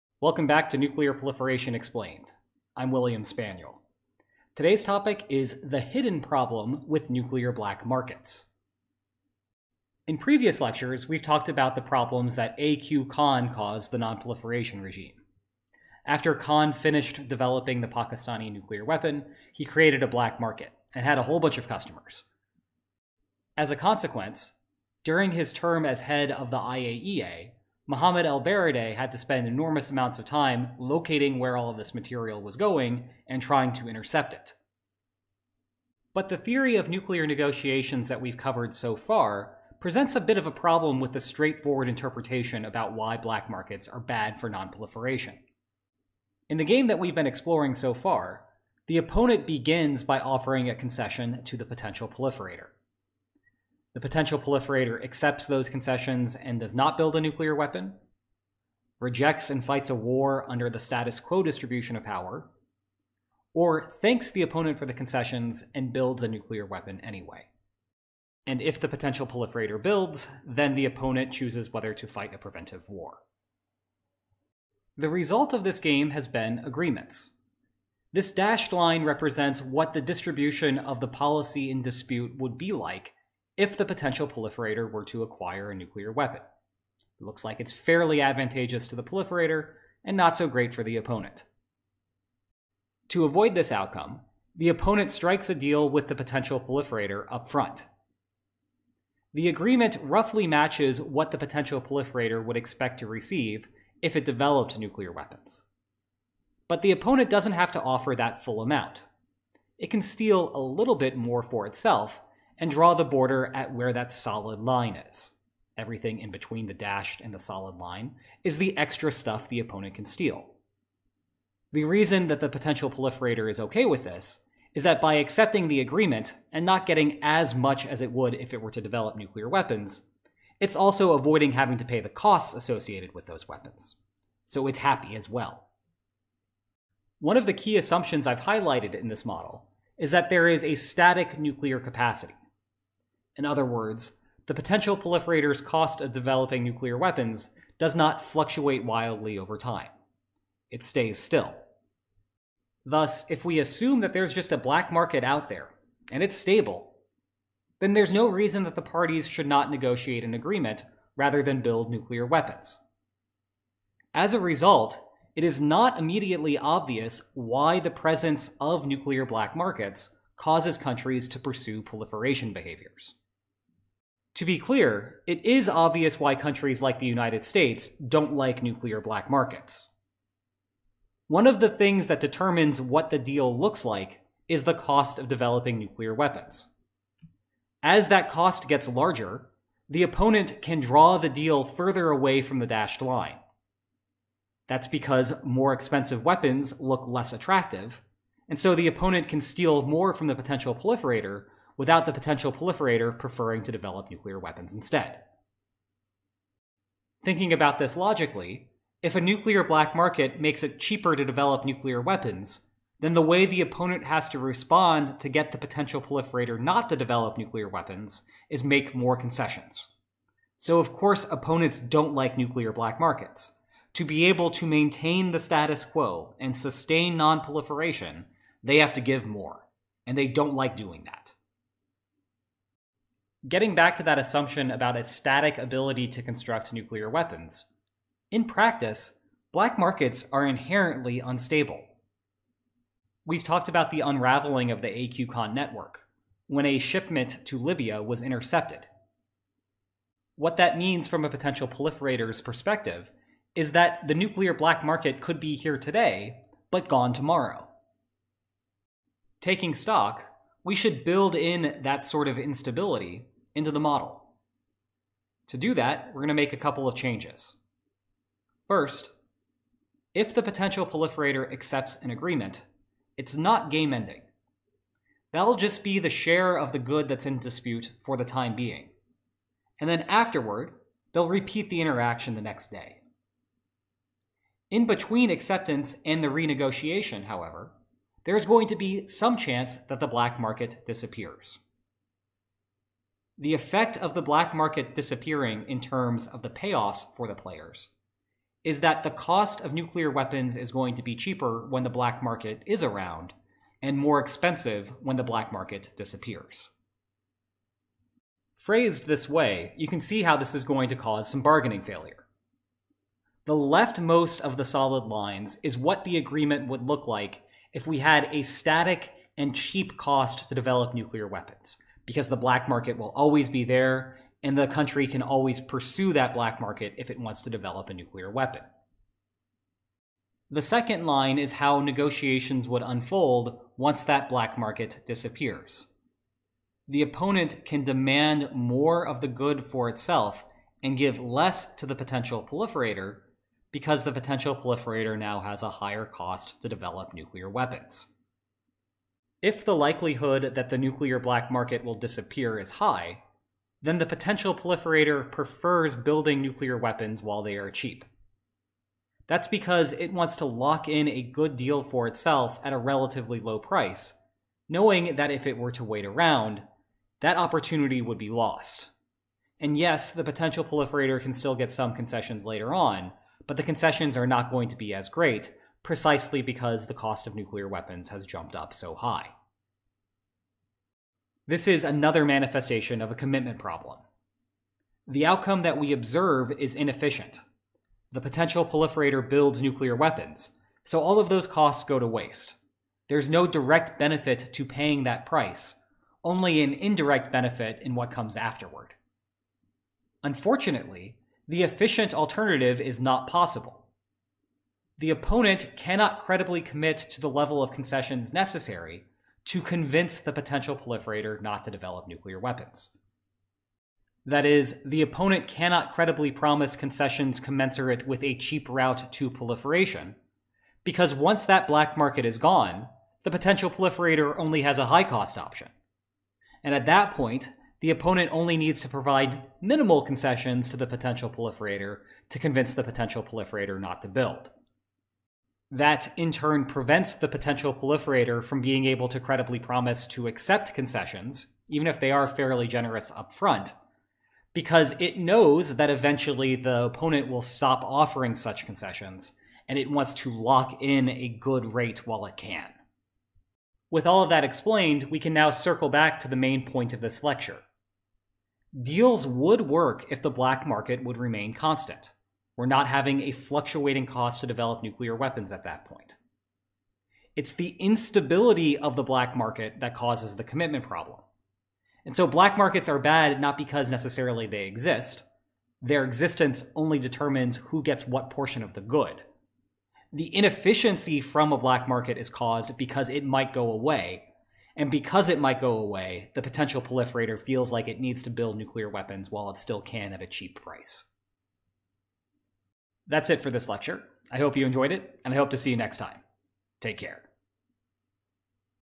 The Hidden Problem of Nuclear Black Markets | Nuclear Proliferation Explained
Nuclear black markets make building weapons cheaper and easier. However, it is not obvious that why they might cause more proliferation. After all, opponents can offer deals commensurate with the potential proliferator's ability to produce nuclear weapons. In a vacuum, the cheaper route just means that the opponent must concede more.

This lecture explains a hidden problem with black markets that can make negotiations fail. Because black markets are inherently unstable, potential proliferators see a need to build nuclear weapons while they can. If they do not---and the black market vanishes---then so will the concessions. As a result, the potential proliferator builds to secure a good deal over the long term.